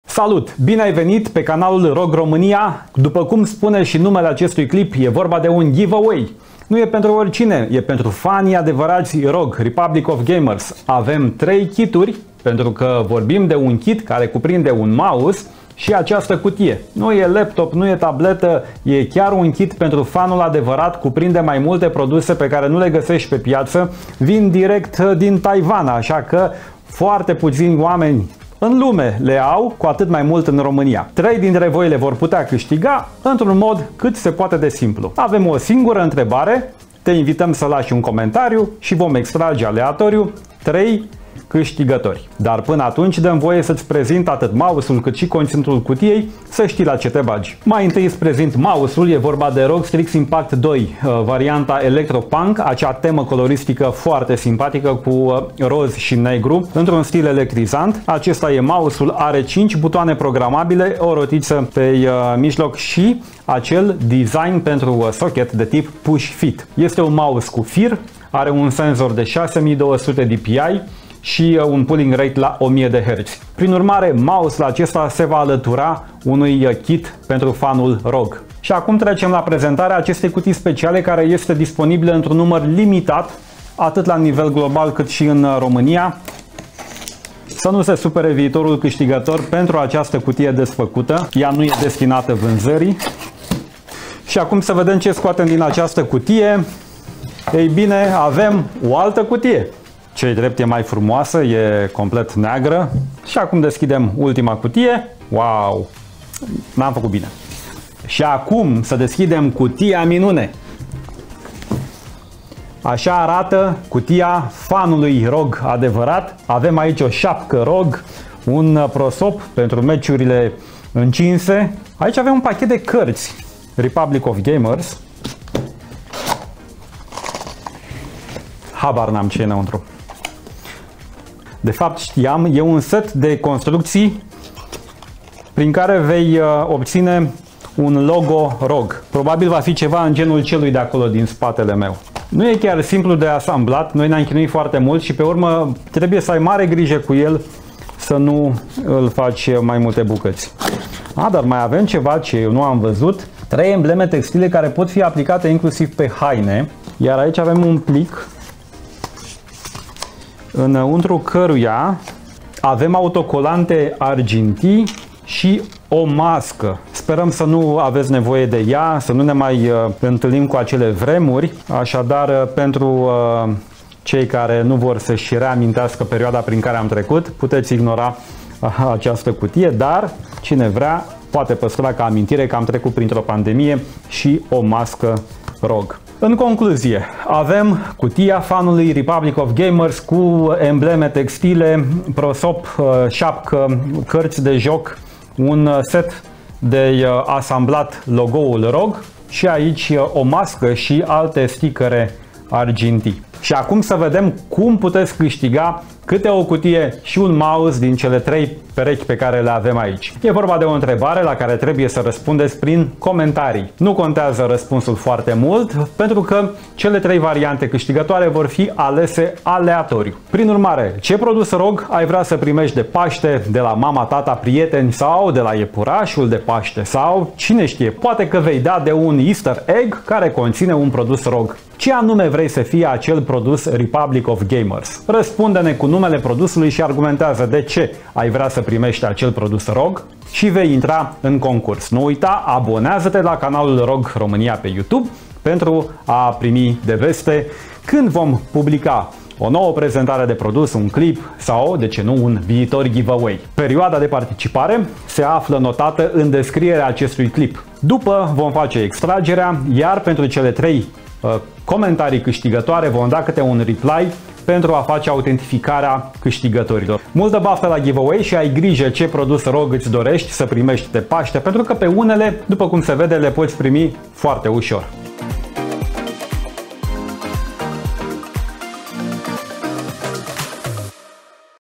[INCHEIAT] ROG Giveaway: Câștigă mouse-ul ROG Strix Impact II + ROG Ultimate Fan Gift Collection
Câștigă unul din cele 3 seturi cuprinzând un mouse ROG Strix Impact II și o cutie ROG Ultimate Fan Gift Collection.

Răspunde la întrebarea: Ce produs Republic of Gamers ai dori să primești de Paște și de ce?
3 comentarii vor fi extrase în mod aleatoriu și vor fi premiate în urma autentificării autorilor. Nu uita să te abonezi la canalul nostru

Perioada de participare: 19 aprilie - 27 aprilie 2022 23:59:59.

UPDATE:
Autorii celor trei comentarii extrase în mod aleatoriu sunt:

Note:
Această tombolă NU are legătură cu platforma YouTube și NU implică Google.
Comentariile multiple vor fi descalificate.
Premiile vor fi acordate prin tragere la sorți.
Datele de identificare ale autorilor comentariilor premiate vor fi colectate în vederea decernării premiilor conform reglementărilor fiscale.
Câștigătorii trebuie să urmeze procedura de autentificare ca autori ai comentariilor extrase.